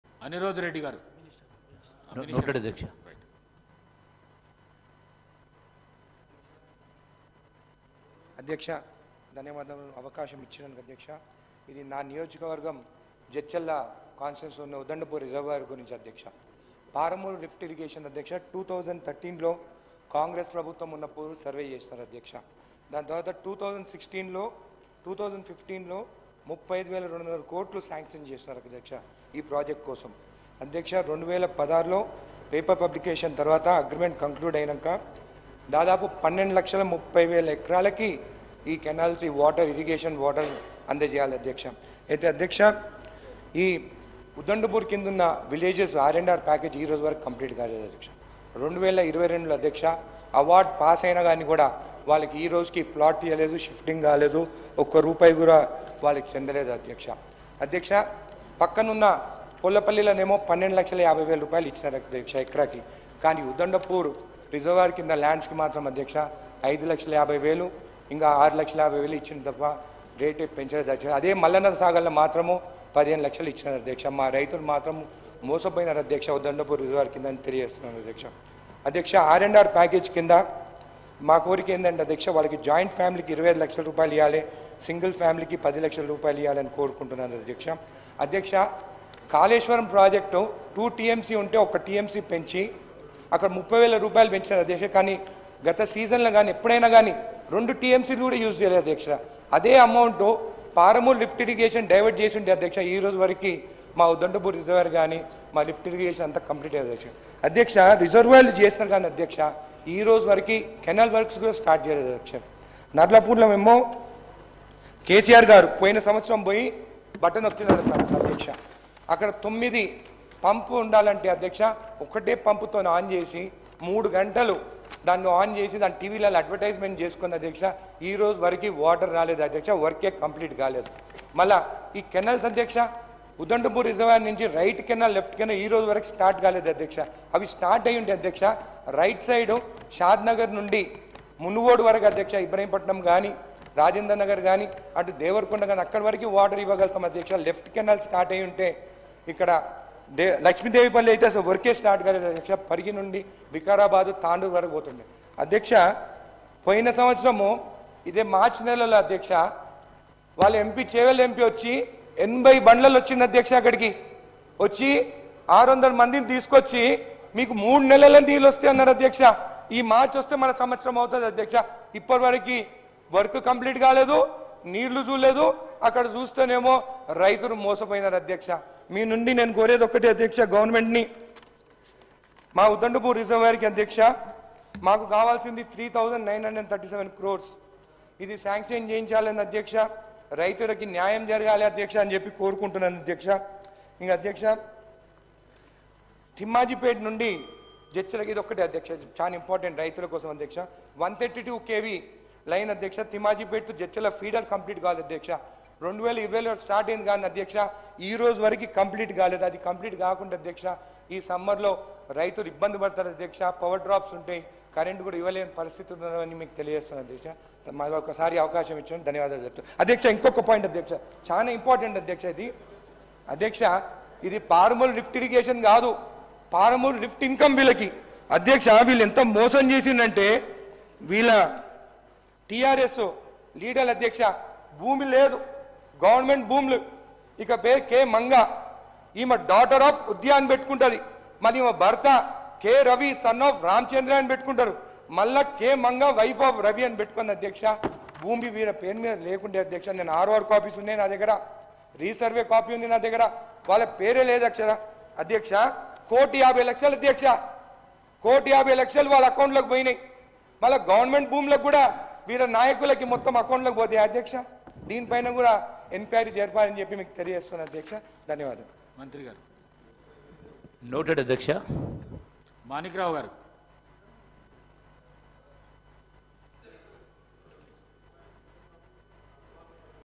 అసెంబ్లీ లో కేసీఆర్ పై రెచ్చిపోయిన అనిరుద్ రెడ్డి.. Congress Anirudh Reddy Fires On KCR | ALO TV
Telangana Assembly LIVE | Telangana Assembly Budget Day 7 | CM Revanth Reddy Vs KCR | YOYO TV News